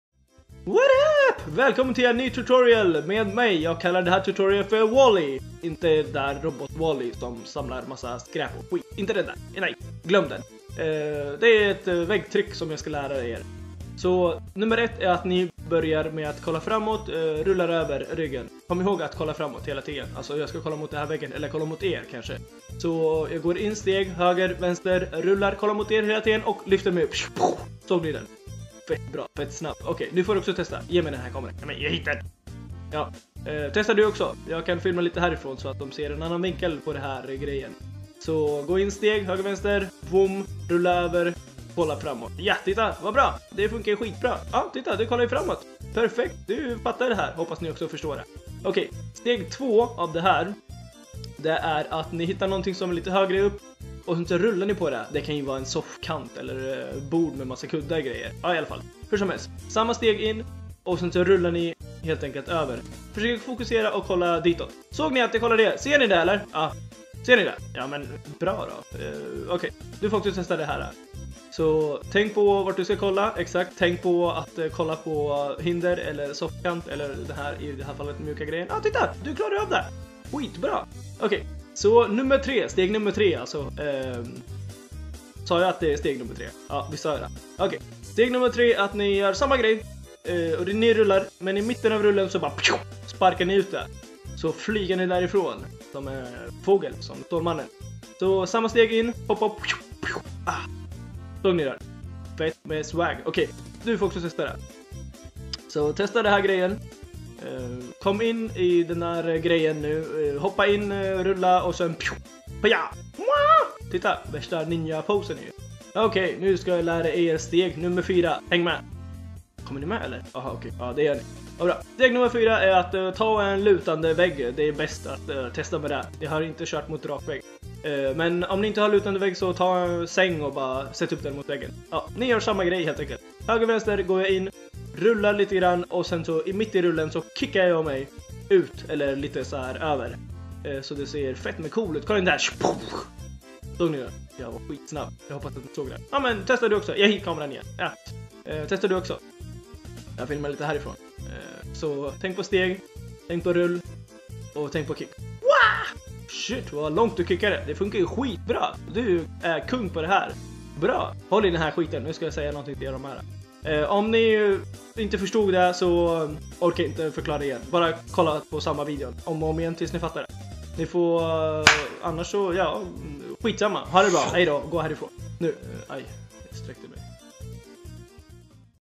Tutorial - WALL-Y!!
HEllo!

Det här är min fett grymma tutorial på Wally... lär er det och lägg till lite style i er parkour rörelse :D